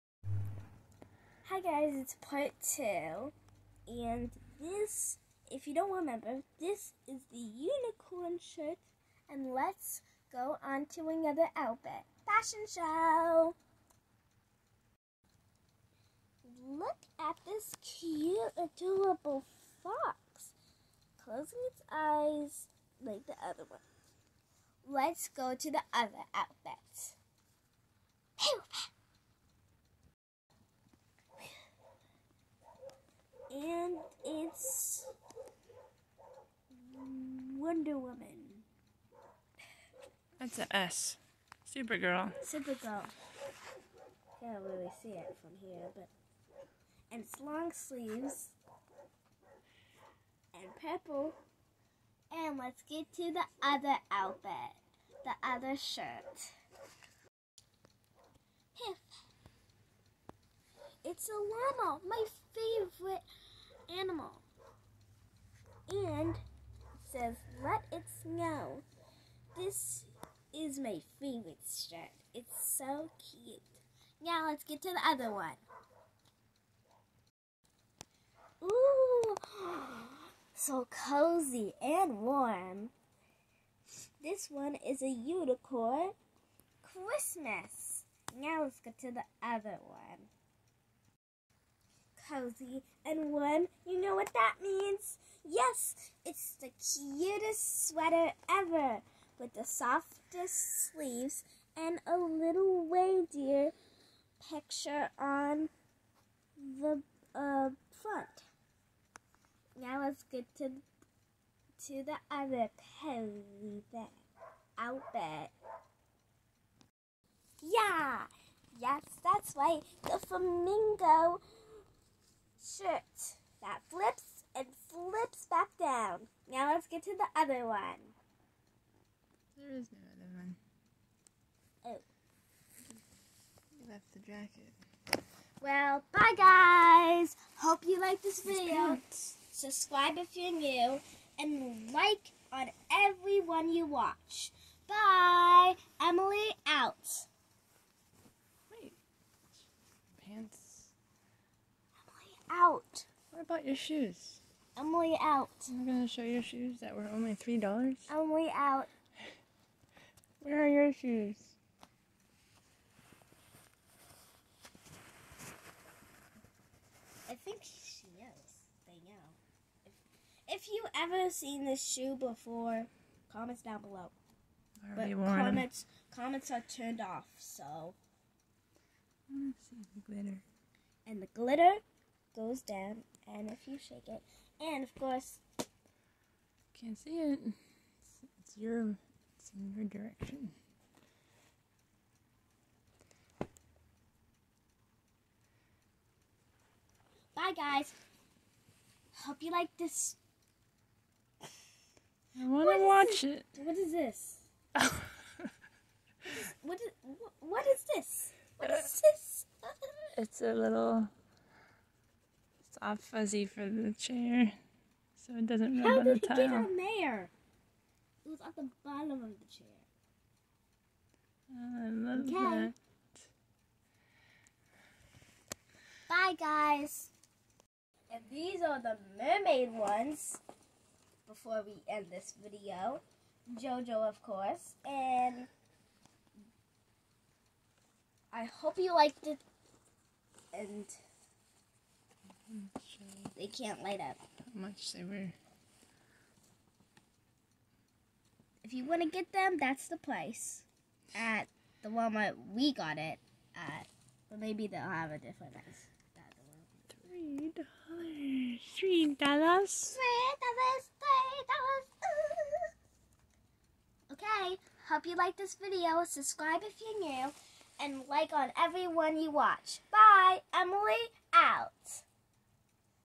B2S clothes part 2
Emily showing her clothes. Part two because the first video got cut off. August 9, 2021